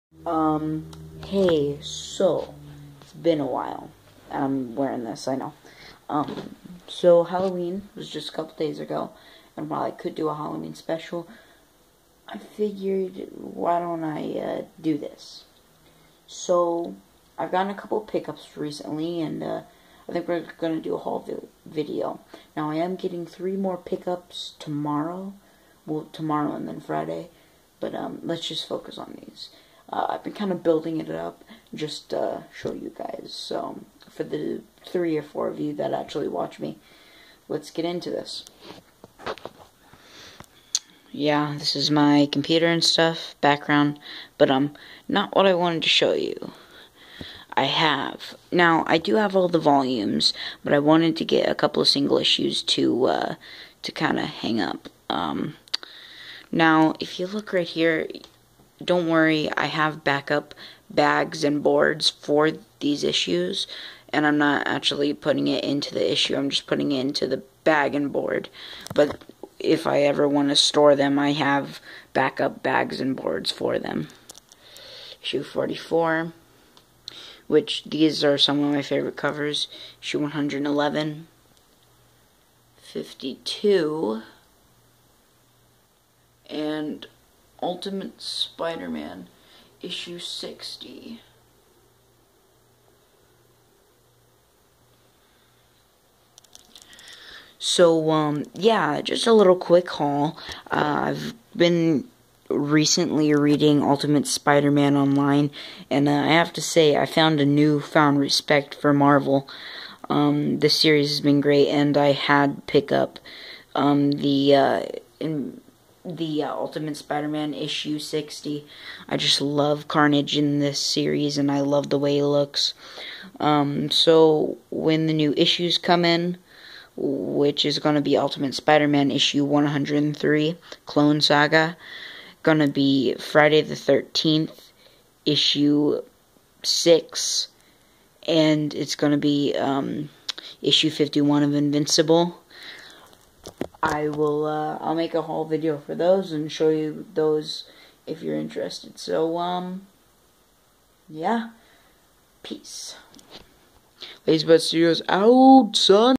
Back issues display update 1
Just showing some of the single issues that I've gotten over the last couple weeks